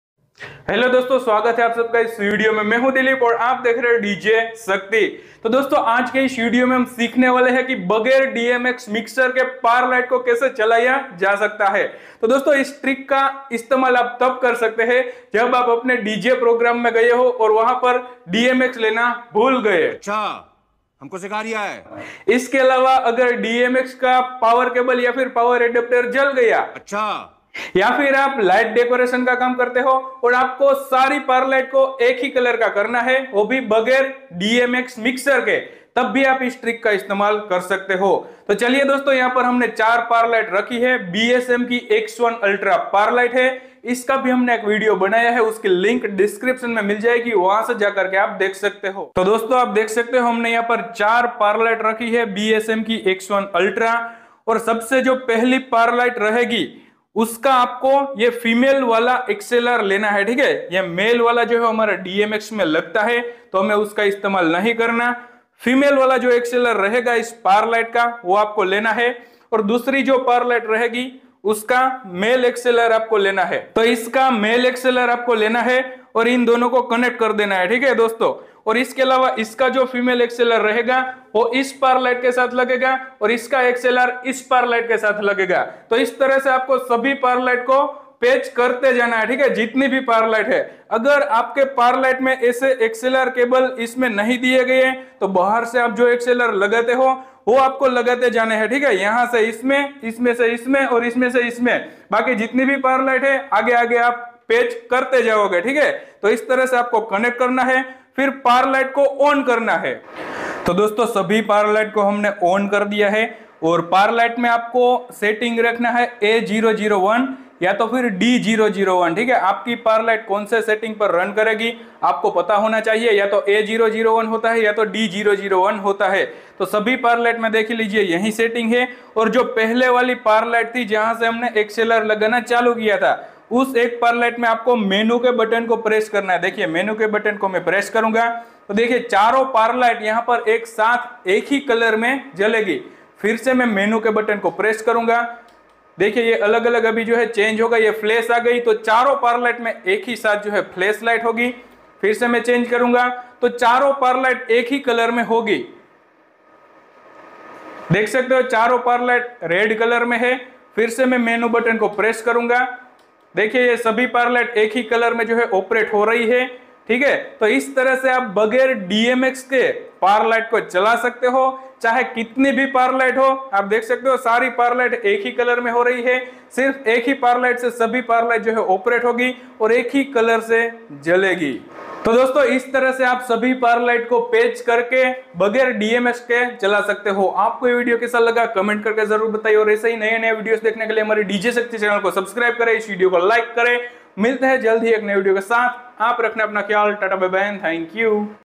बगैर DMX मिक्सर के Parr Light चलाना सीखिए | Disco Light | DMX 512 Mixer | BSM X1
Shop Name :- Shakti Electronics
Shop Address :- Gondal, Gujarat, 360311

Hello Dosto, Humne is video Mai sikhaya hai ki DMX 512 mixer ke bager Parr light ko kaise chalaya ja skta hai umeed karta hun apko ye video acha laga hoga

❤️ Keep Love

Take Care 😊